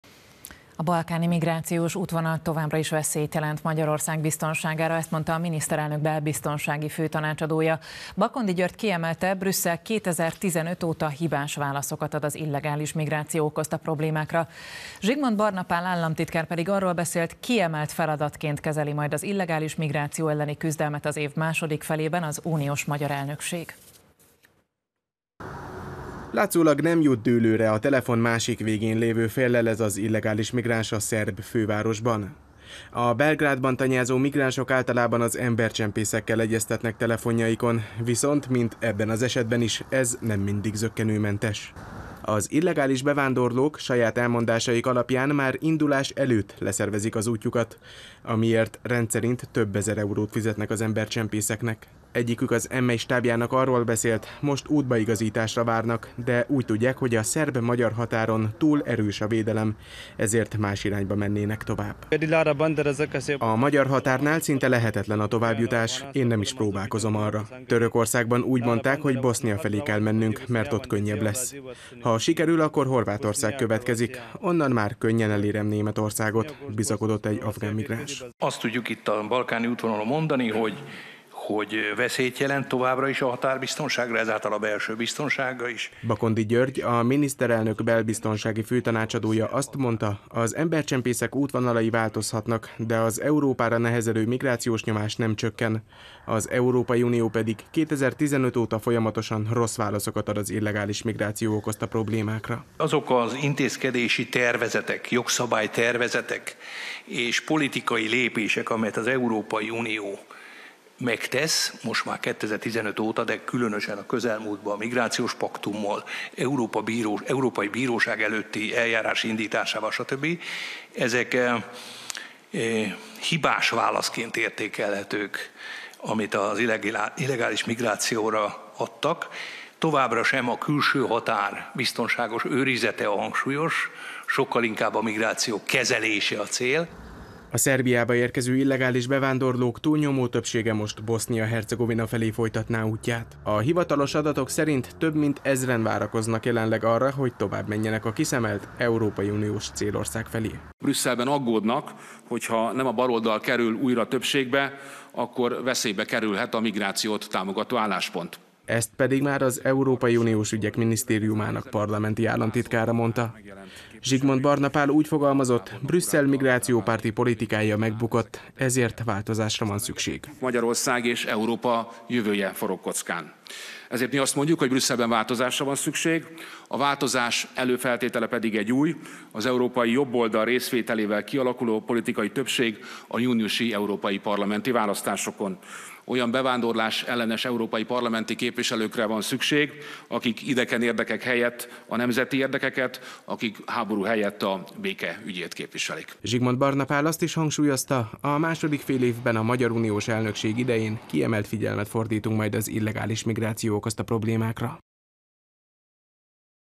Bakondi György: az Európai Unió hibás válaszokat ad az illegális migrációra
A balkáni migrációs útvonal továbbra is veszélyt jelent Magyarország biztonságára - ezt mondta a miniszterelnök belbiztonsági főtanácsadója. Bakondi György kiemelte: Brüsszel 2015-óta hibás válaszokat ad az illegális migráció okozta problémákra. Zsigmond Barna Pál államtitkár pedig arról beszélt: kiemelt feladatként kezeli majd az illegális migráció elleni küzdelmet az év második felében, az uniós magyar elnökség.